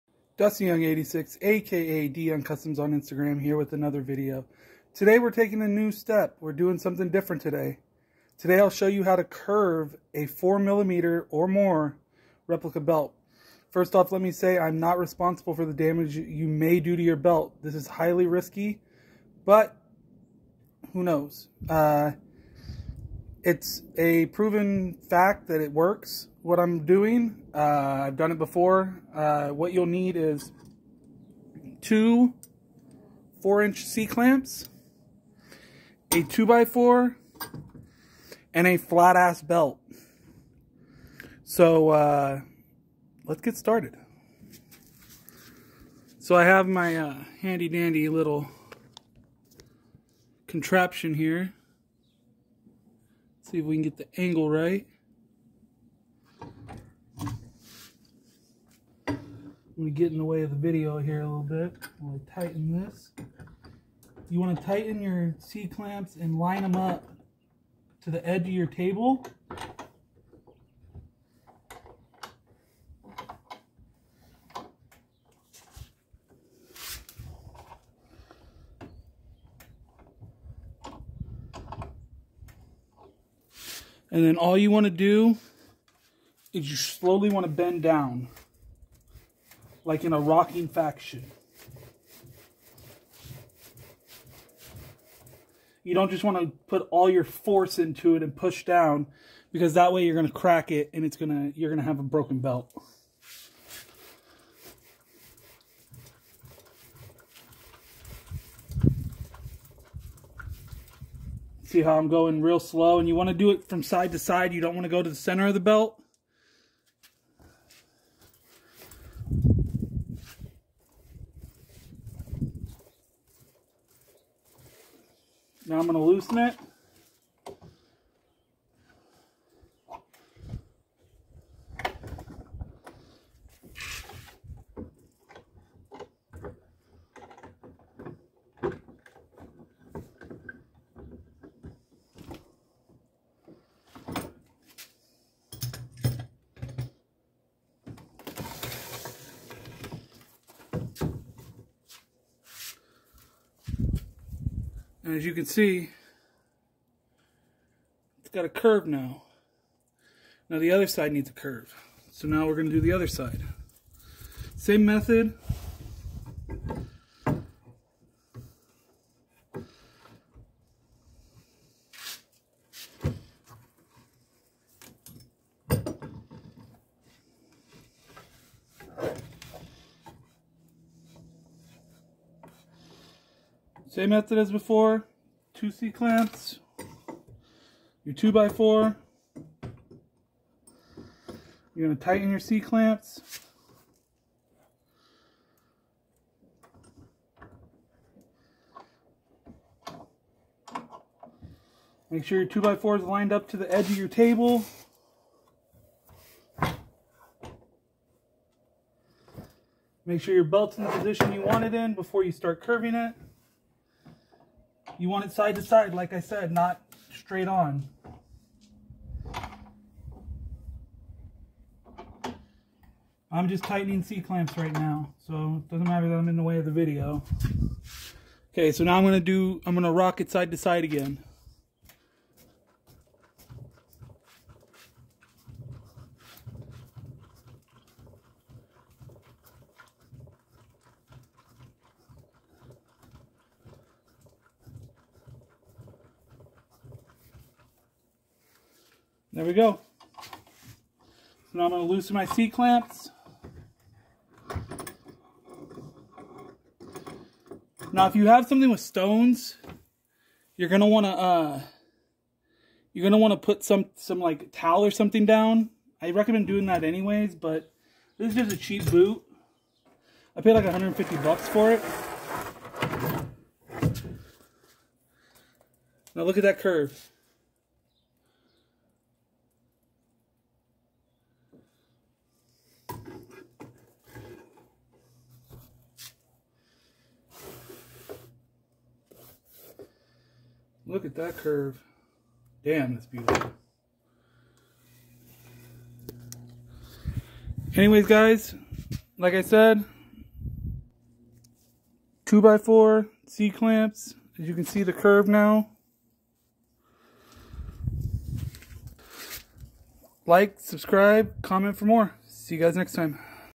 How to Curve a 4mm and Above Replica Belt.
In this video I will show you how to Curve your WWE shop and other 4mm, 5mm and even 6mm replica belts.